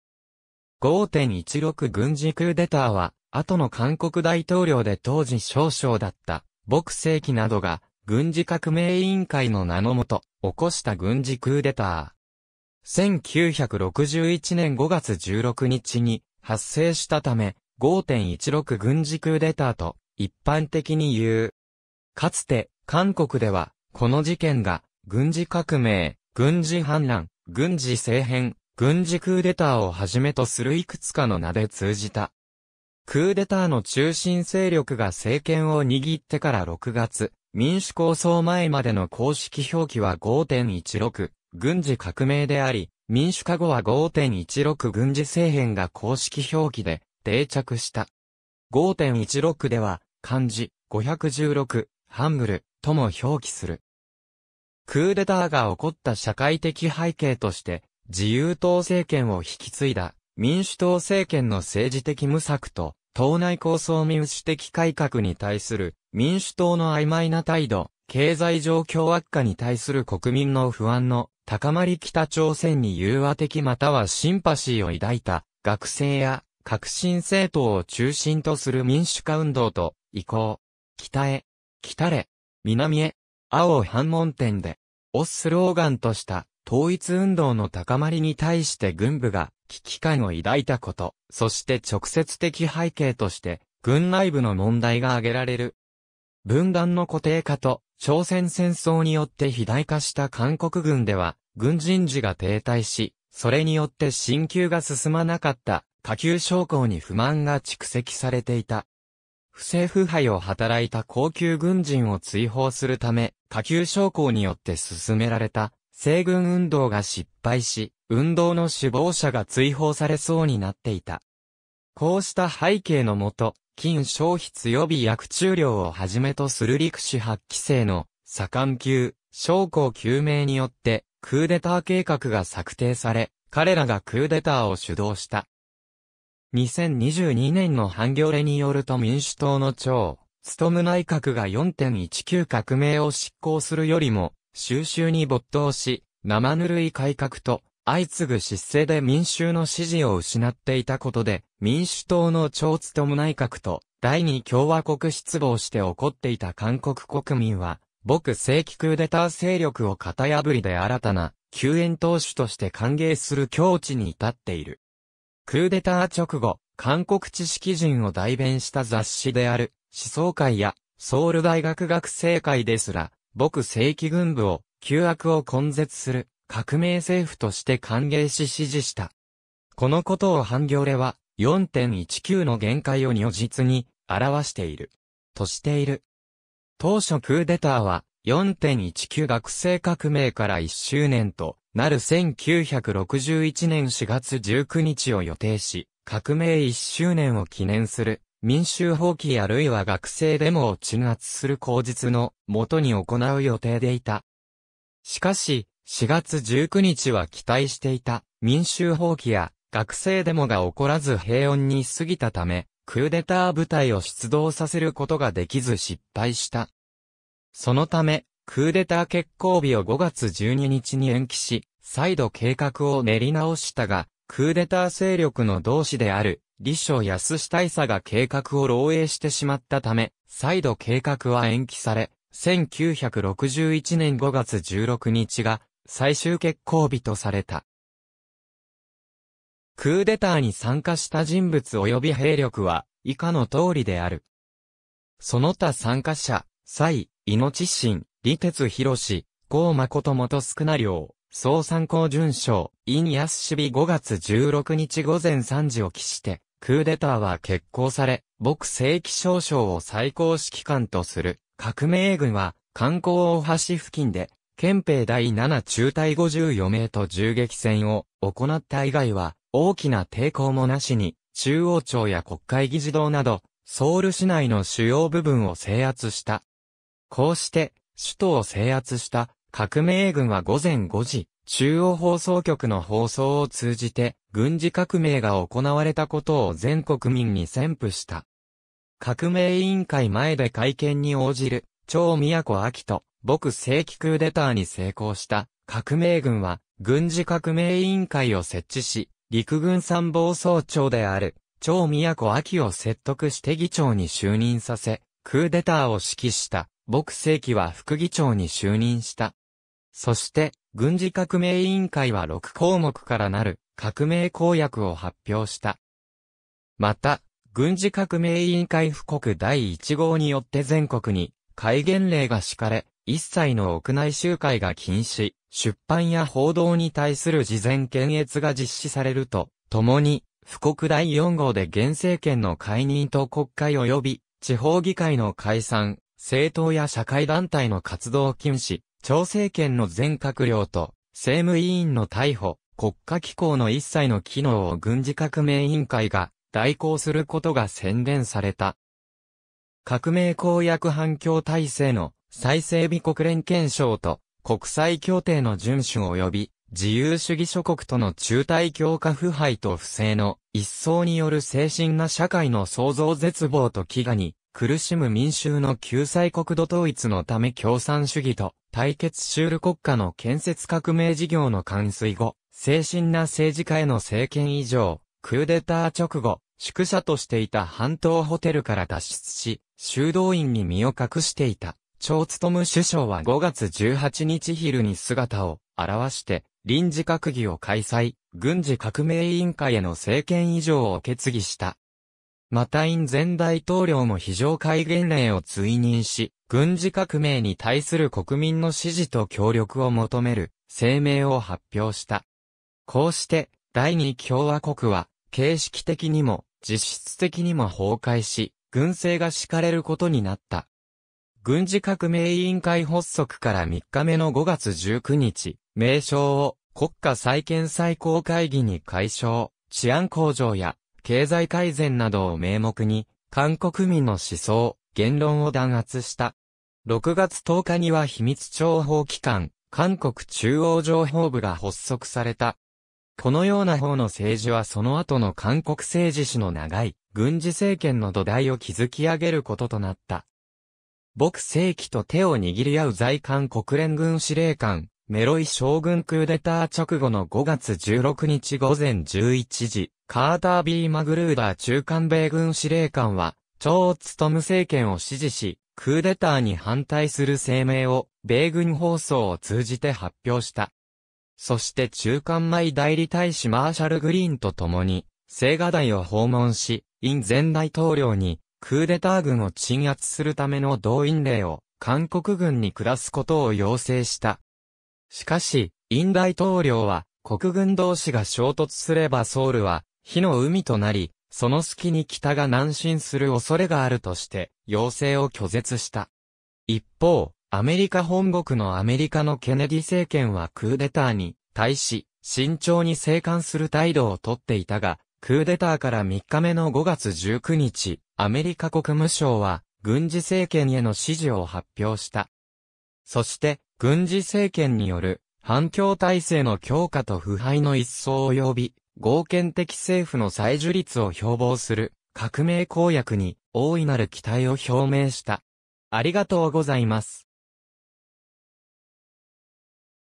5・16軍事クーデター
5・16軍事クーデター（5・16ぐんじクーデター）は、後の韓国大統領で当時少将（第2野戦軍副司令官）だった朴正煕などが軍事革命委員会の名の下、起こした軍事クーデター。
1961年5月16日に発生したため「5・16軍事クーデター」と一般的に言う。
かつて韓国では、この事件が軍事革命（保守側の呼称）、軍事叛乱（進歩側の呼称）、軍事政変、軍事クーデターをはじめとするいくつかの名で通じた。
クーデターの中心勢力が政権を握ってから6月民主抗争前までの公式表記は5・16軍事革命であり、民主化後は5・16軍事政変が公式表記で定着した。
「5・16」では、漢字「五一六」、ハングル「오일륙」とも表記する。
クーデターが起こった社会的背景として、 自由党政権を引き継いだ民主党政権の政治的無策と党内抗争 民主的改革に対する民主党の曖昧な態度、経済状況悪化に対する国民の不安の高まり 北朝鮮に融和的又はシンパシーを抱いた学生や革新政党を中心とする民主化運動と「行こう！北へ！来たれ！南へ！会おう板門店で！」をスローガンとした統一運動の高まりに対して軍部が危機感を抱いたこと、そして直接的背景として軍内部の問題が挙げられる。
分断の固定化と朝鮮戦争によって肥大化した韓国軍では軍人事が停滞し、それによって進級が進まなかった下級将校に不満が蓄積されていた。
不正腐敗を働いた高級軍人を追放するため下級将校によって進められた「整軍運動」が失敗し、運動の首謀者が追放されそうになっていた。
こうした背景の下、金鍾泌予備役中領（中佐）を初めとする陸士8期生の佐官級将校9名によってクーデター計画が策定され、彼らがクーデターを主導した。
2022年のハンギョレによると民主党の張勉内閣が4.19革命を執行するよりも収拾に没頭し、生ぬるい改革と相次ぐ失政で民衆の支持を失っていたことで、民主党の張勉内閣と第2共和国失望して怒っていた韓国国民は、「朴正煕クーデター勢力」を型破りで新たな救援投手として歓迎する境地に至っている。
クーデター直後、韓国知識人を代弁した雑誌である「思想界」や、ソウル大学学生会ですら、朴正煕軍部を「旧悪を根絶する」「革命政府」として歓迎し支持した。
このことをハンギョレは、「4・19（革命）の限界を如実に表している。」としている。
当初クーデターは、4・19学生革命から1周年となる1961年4月19日を予定し、革命1周年を記念する民衆蜂起或いは学生デモを鎮圧する口実の下に行う予定でいた。
しかし、4月19日は期待していた民衆蜂起や学生デモが起こらず平穏に過ぎたため、クーデター部隊を出動させることができず失敗した。
そのため、クーデター決行日を5月12日に延期し、再度計画を練り直したが、クーデター勢力の同志である李鍾泰大佐が計画を漏洩してしまったため、再度計画は延期され、1961年5月16日が最終決行日とされた。
クーデターに参加した人物および兵力は以下の通りである。
その他参加者：蔡命新（第5師団長）、李哲熙（陸軍防諜部隊長）、康誠元少領（陸軍情報参謀本部企画官）、宋贊鎬准将（高射砲旅団長）、尹泰日 5月16日午前3時を期してクーデターは決行され、朴正煕少将を最高指揮官とする革命軍は、漢江大橋付近で憲兵第7中隊（中隊長：金錫律大尉）50余名と銃撃戦を行った以外は、大きな抵抗も無しに中央庁や国会議事堂などソウル市内の主要部分を制圧した。
こうして首都を制圧した革命軍は午前5時、中央放送局の放送を通じて軍事革命が行われたことを全国民に宣布した。
革命委員会前で会見に応じる張都暎（左）と朴正煕（右） クーデターに成功した革命軍は軍事革命委員会を設置し、陸軍参謀総長である張都暎を説得して議長に就任させ、クーデターを指揮した朴正煕は副議長に就任した。
そして軍事革命委員会は六項目からなる革命公約を発表した。
また軍事革命委員会布告第一号によって全国に戒厳令が敷かれ、一切の屋内集会が禁止、出版や報道に対する事前検閲が実施されるとともに、布告第四号で現政権（張勉政権）の解任と国会及び地方議会の解散、政党や社会団体の活動禁止、張政権の全閣僚と政務委員の逮捕、国家機構の一切の機能を軍事革命委員会が代行することが宣言された。
革命公約 反共体制の再整備 国連憲章と国際協定の遵守および自由主義諸国との紐帯強化 腐敗と不正の一掃による清新な社会の創造 絶望と飢餓に苦し...